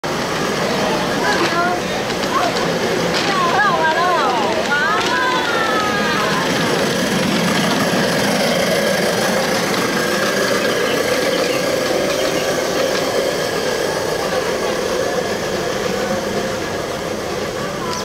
2015.6.8 阿里山森林鐵路 北門站 林松雄老師 阿里山森林鐵路14號蒸汽火車模型 動態行駛
2015.6.8 阿里山森林鐵路 北門站 林松雄老師 阿里山森林鐵路14號蒸汽火車模型 動態行駛